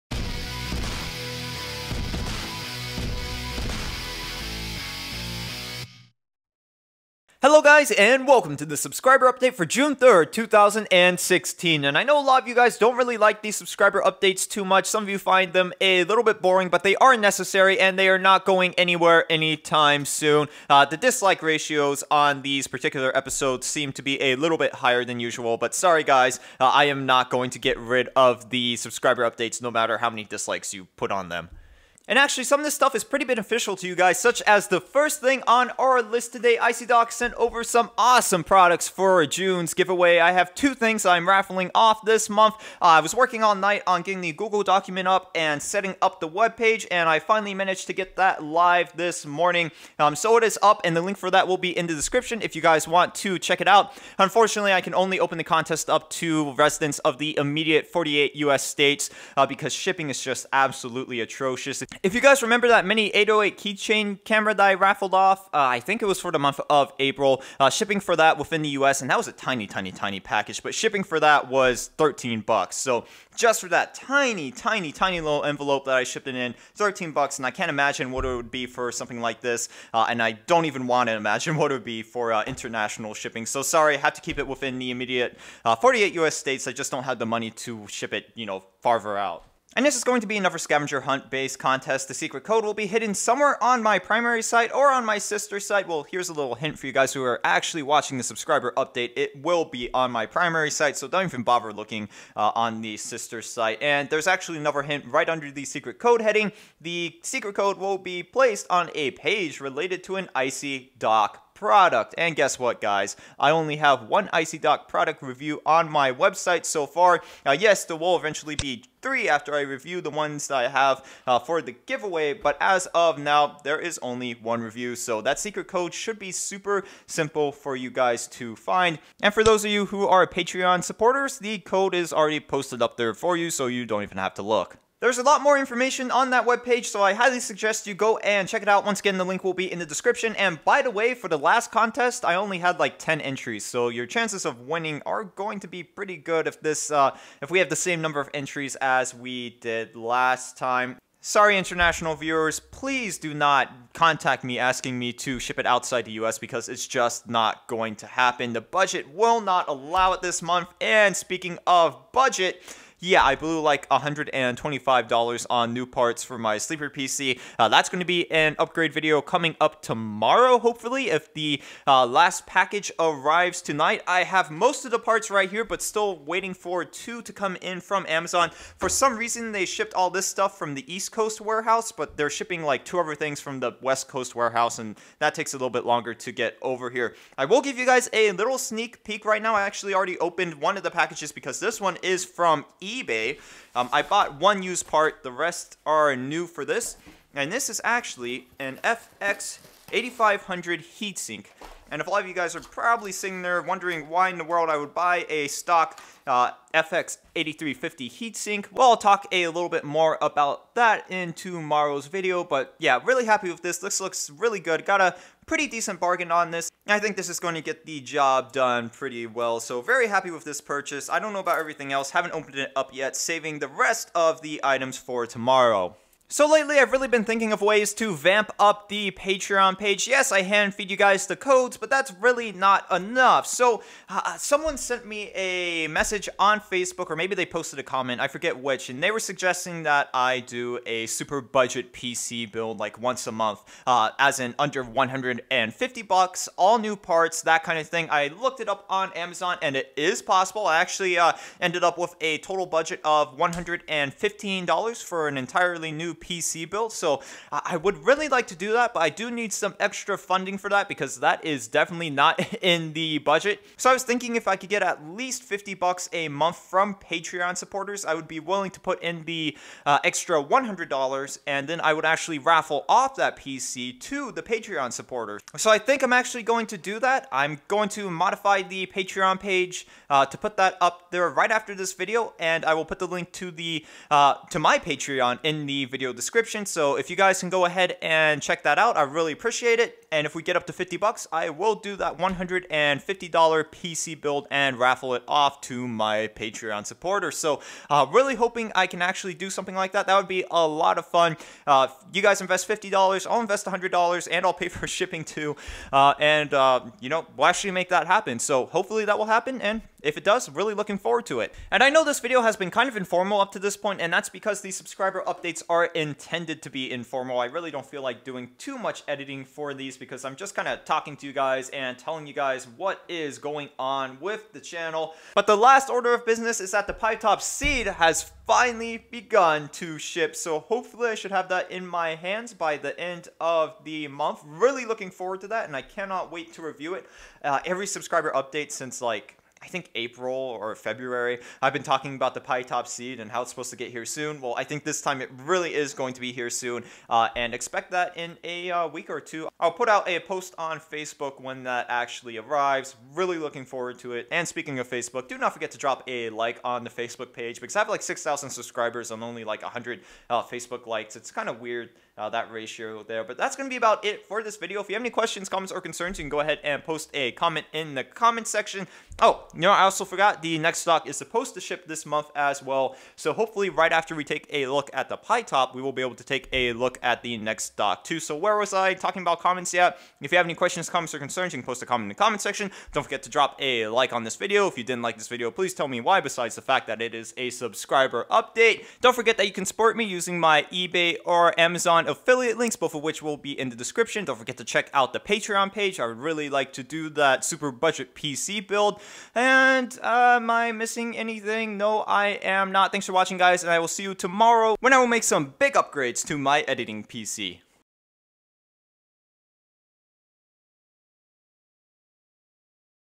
Subscriber Update 6/3/2016: June ICY DOCK Giveaway, PC upgrades, $150 Patreon PC build, Pi-top
In today's subscriber update I will talk about this month's giveaway,  the upgrades I have in store for my editing PC, and the fact that the Pi-top CEED is finally shipping.  I will also speculate the possibility of building a $150 super budget PC for my patrons.